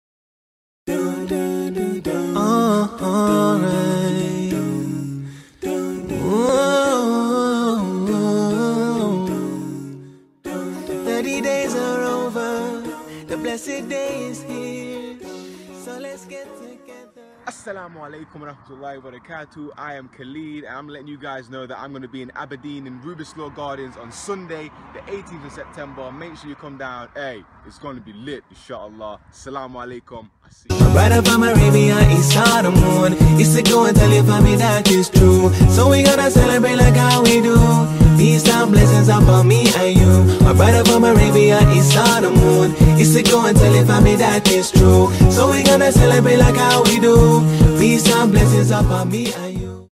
EITG Khaled Promo Final HD
Khaled Siddique is coming to Aberdeen on September 18th 2016 for our united Eid celebration Eid in the Garden!

Come join us for some family fun whilst you browse through delicious food stalls, clothes markets and henna stalls. This is a day not to be missed!

Eid In the Garden
18th September 2016
12pm to 5pm
Rubislaw Terrace Gardens,
Albyn Pl, Aberdeen, AB10 1XE